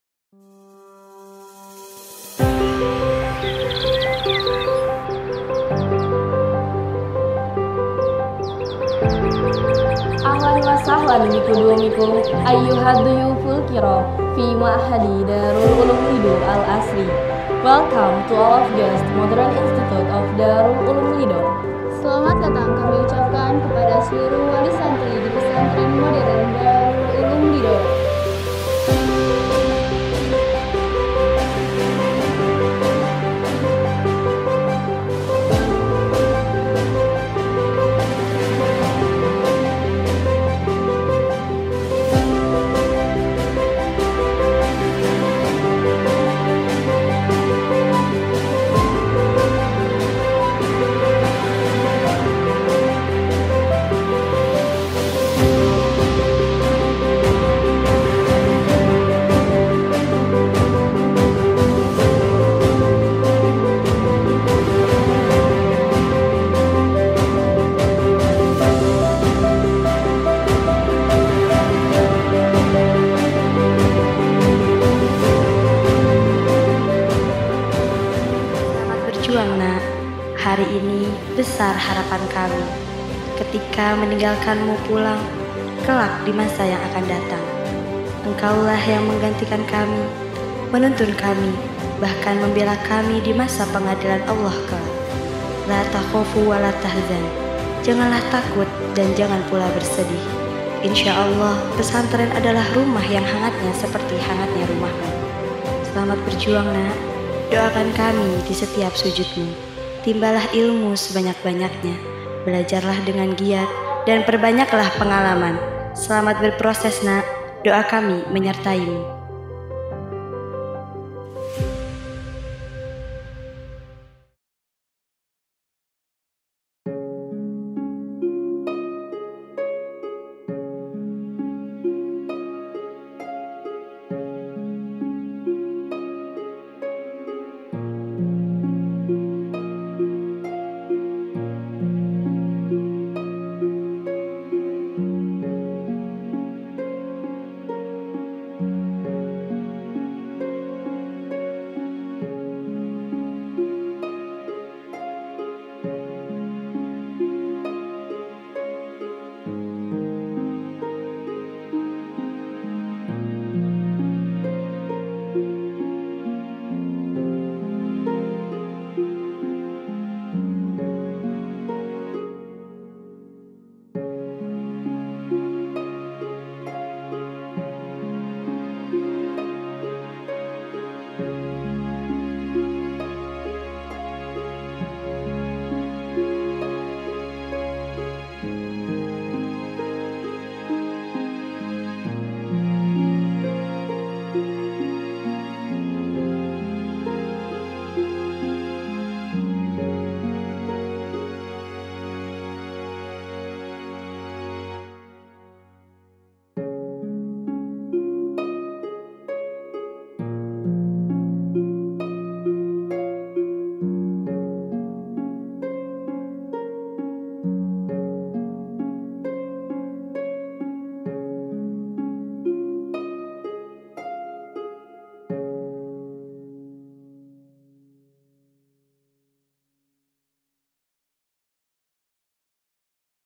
Kedatangan Santri Baru 2023 (After Movie)
Selamat datang santri baru di pesantren modern daarul 'uluum lido.
selamat berjuang untuk belajar, timbalah ilmu sebanyak-banyaknya, belajarlah dengan giat dan perbanyaklah pengalaman,selamat berproses...
Semangat...